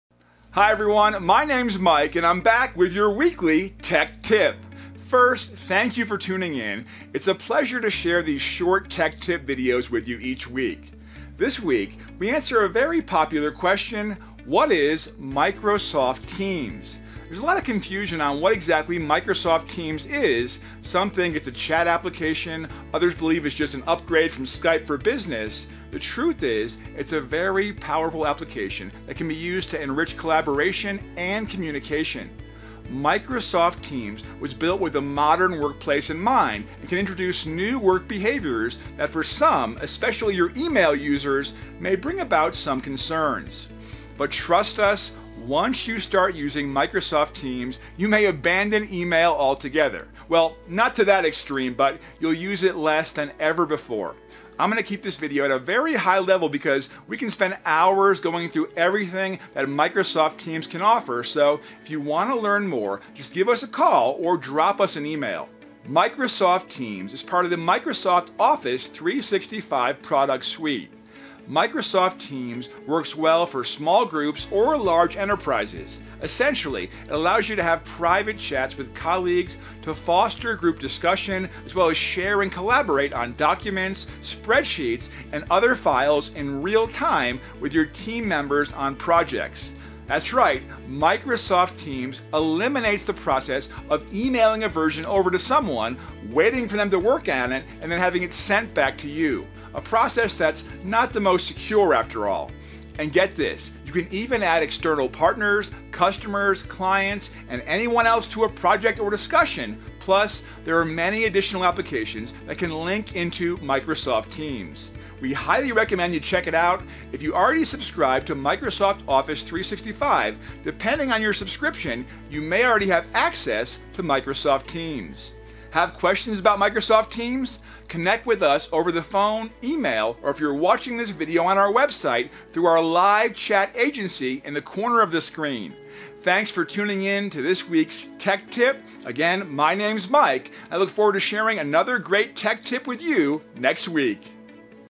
What Is Microsoft Teams?
What is Microsoft Teams?  Microsoft Teams is part of the Microsoft Office 365 Suite of products.  Want this video to help market your managed IT services business?